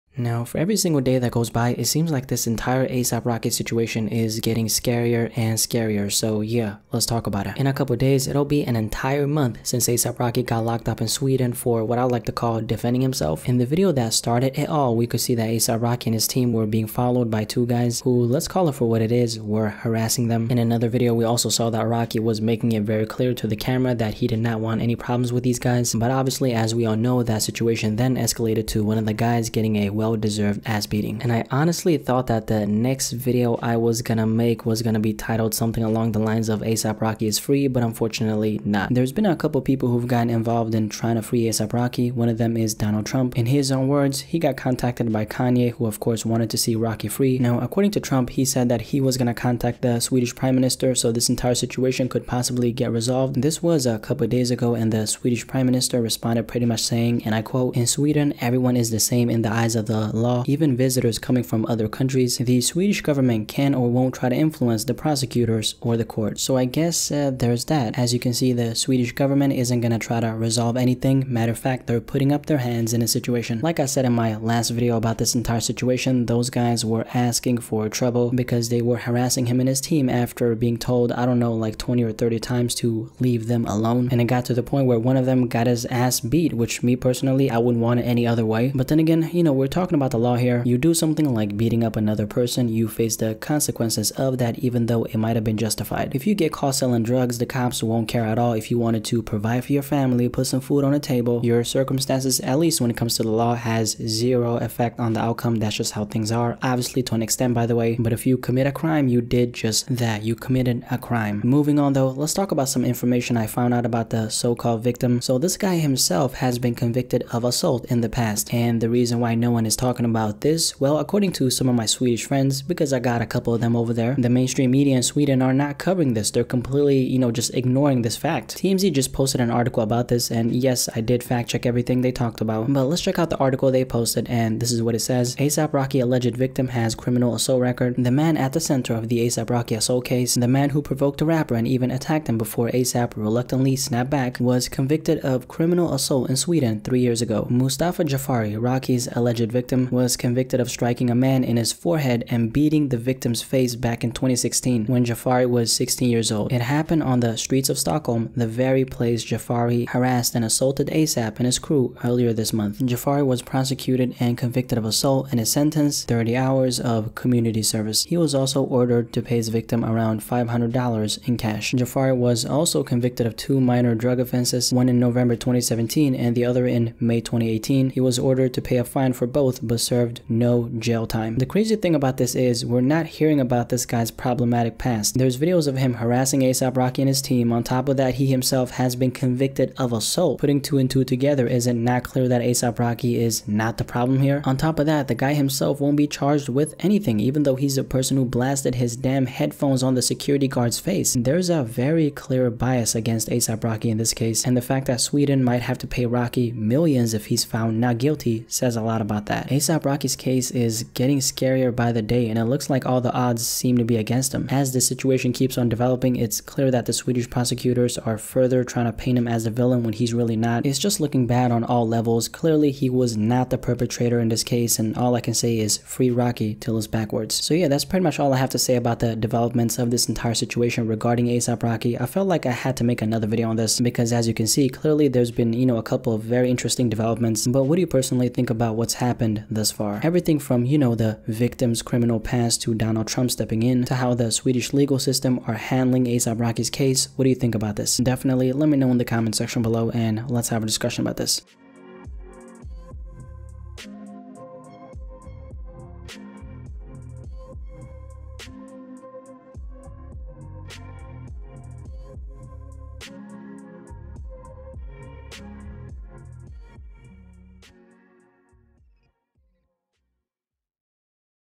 A$AP Rocky's Situation Is Getting Scarier
A$AP Rocky's Situation Is Getting Scarier - Now, in a couple of days it'll be an entire month since A$AP Rocky got locked up in Sweden for what went down with him, his team, and two other guys. Donald Trump & Kanye West have both gotten involved in the release of Rocky, but it still seems like all the odds are against him. That's what I talk about.

In this video I talk about A$AP Rocky.